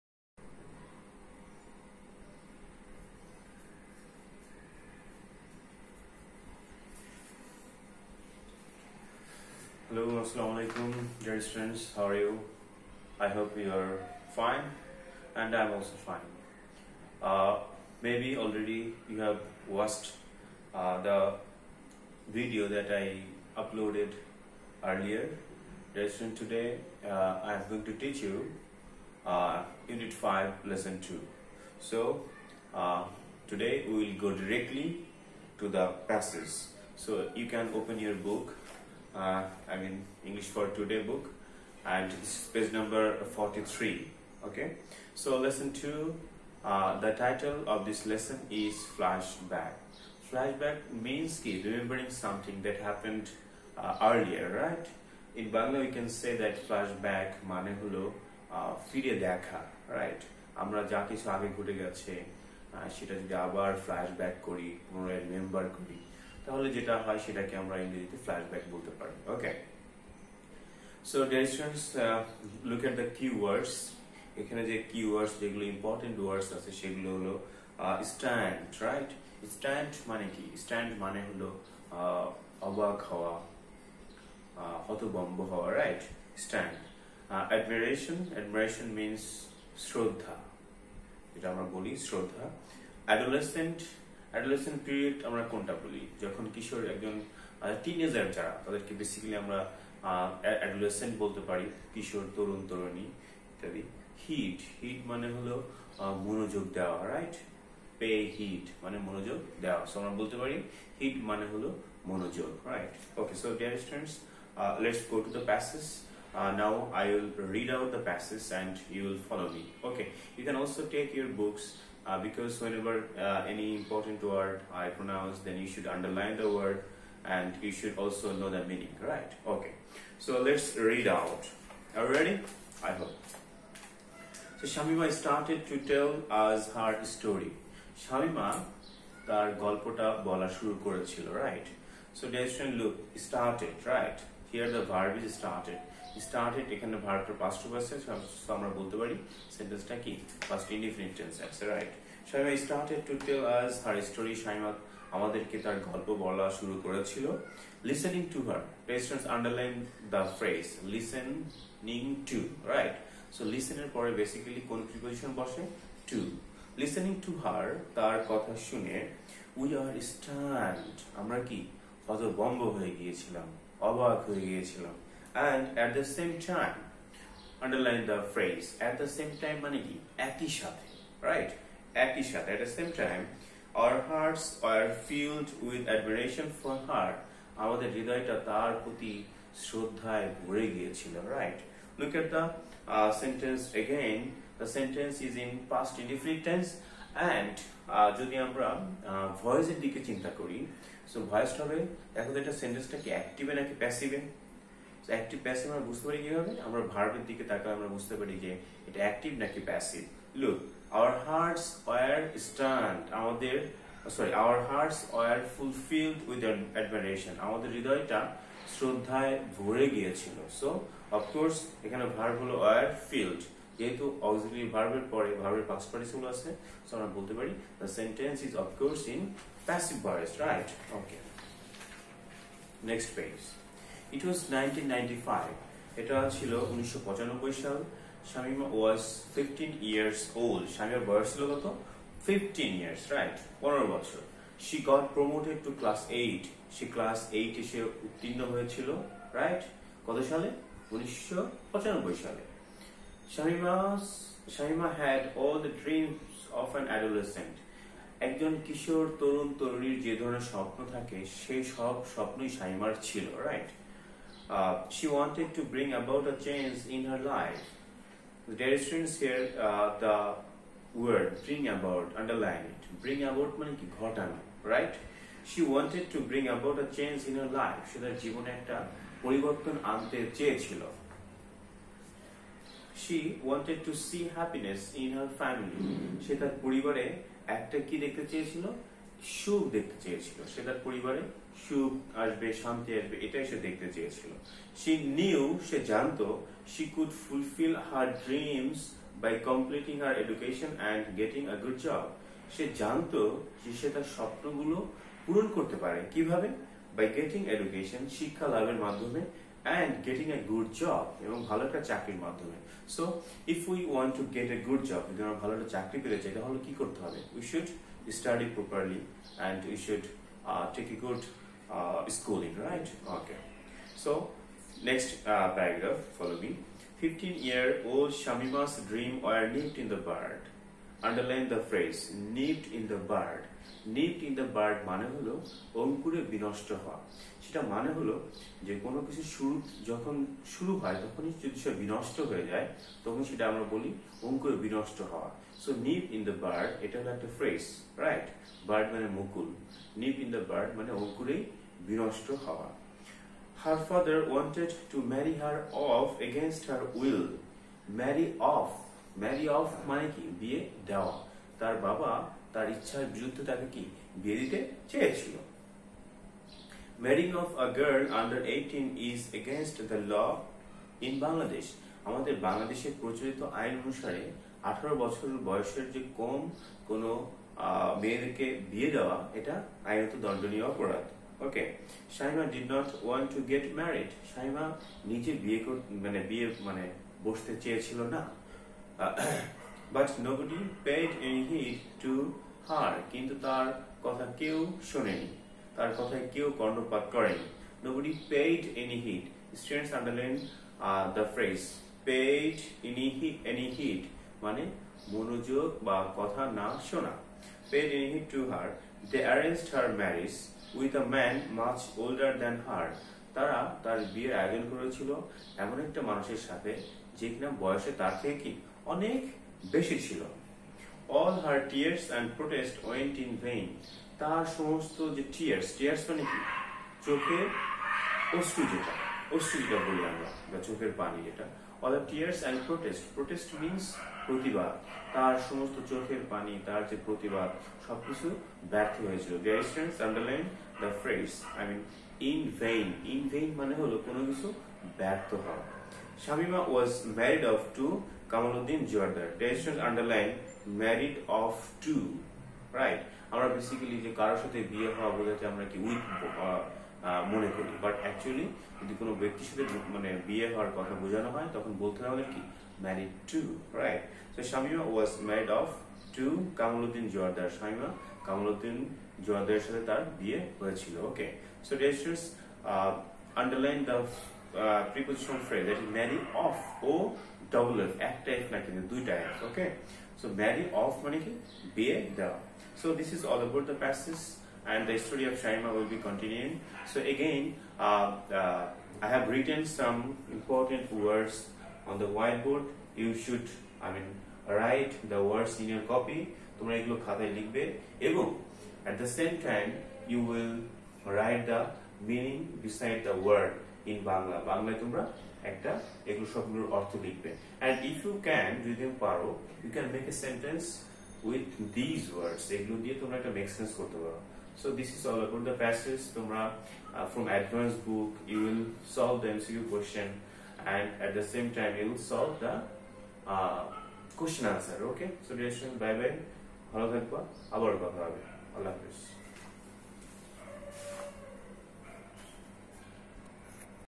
English For Today | Class 8 | Unit 5 | Lesson 2
English For Today | Class 8 | Unit 5 | Lesson 1
Instructor  : Mr. Jahid Hasan

'করনা'-র কারনে তোমাদের পড়াশুনার যাতে কোন ক্ষতি না হয়  তাই আমাদের এই প্রচেষ্ঠা। বাইরে বের হওয়ার কোনো উপায় না থাকায় স্টুুুুডিও ব্যবহার না করতে পারায় ভিডিও দেখতে একটু সমস্যা হতে পারে। আশা করি পরবর্তীতে আরও  ভাল মানের ভিডিও তোমাদের  জন্য  আনা হবে।